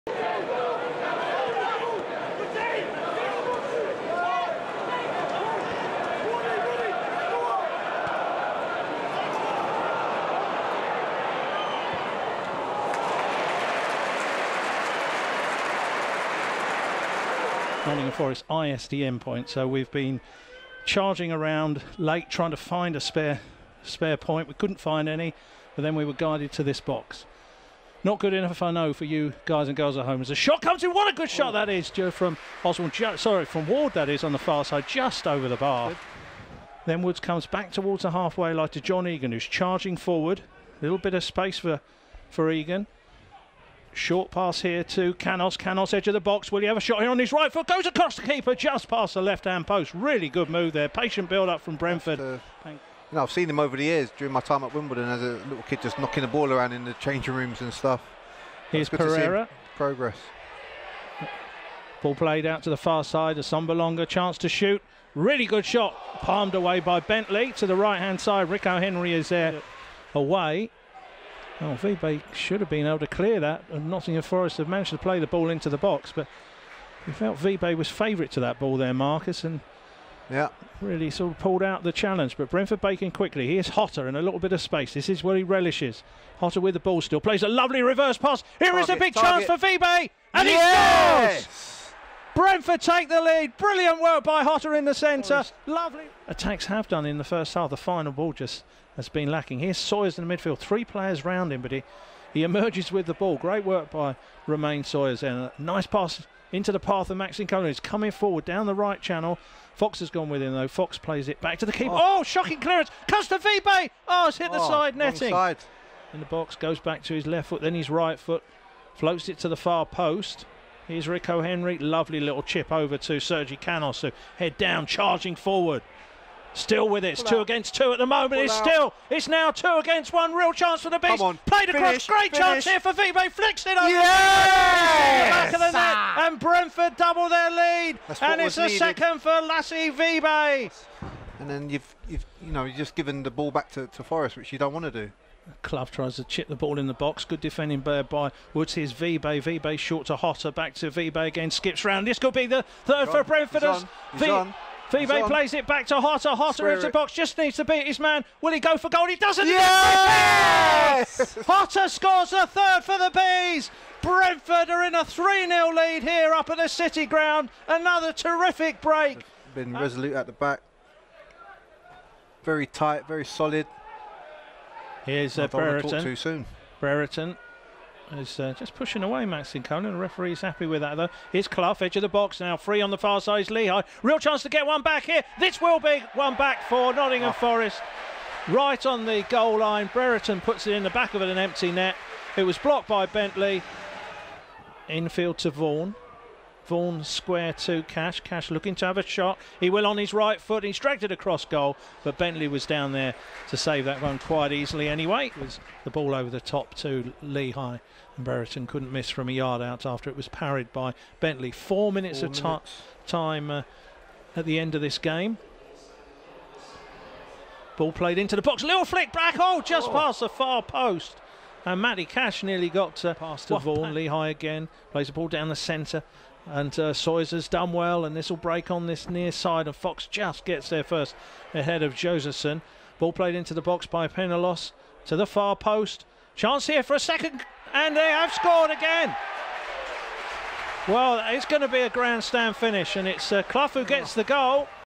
Match Highlights: Nottingham Forest 2 Brentford 3
Lasse Vibe nets and provides while Jota provides and nets as Brentford continue unbeaten run.